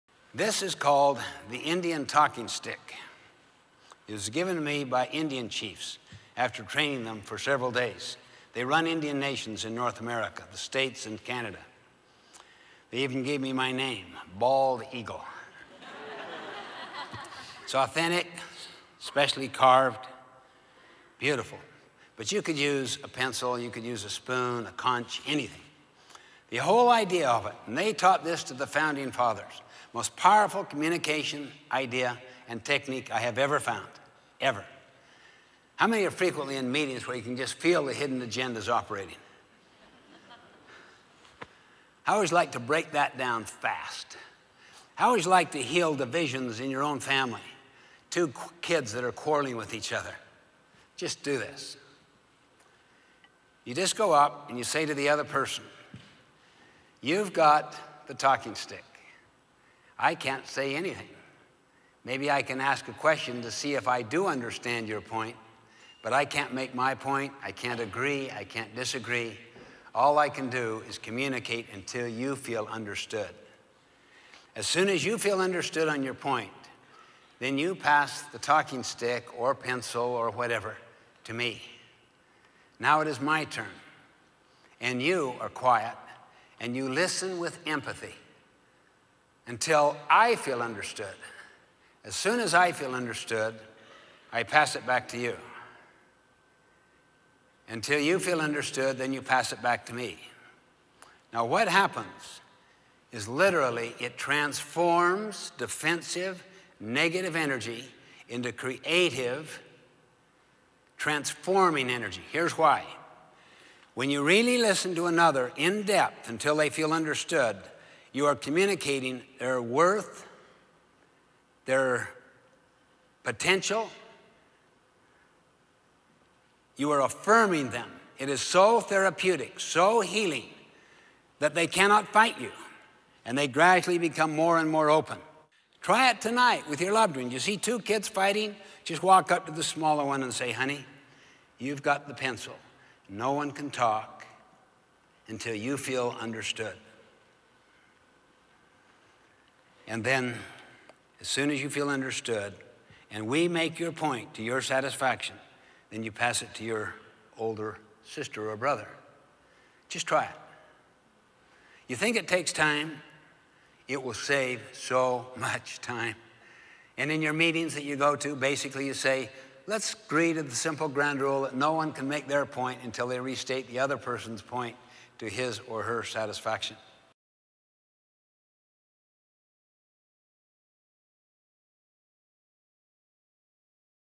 Stephen Covey: Indian Talking Stick
Dr. Stephen Covey: The 8th Habit. Stephen Covey defined the 7 habits of effective leaders more than a decade ago. Now ... The 8th Habit is revealed! In this compelling presentation, Dr. Covey pushes leaders to be not only effective but truly great. Learn from the most respected source on leadership how to: Find your voice and inspire others to find theirs; Contribute and succeed in the Knowledge Economy; Make leadership a choice versus a position; Turn an organization's top goals into action.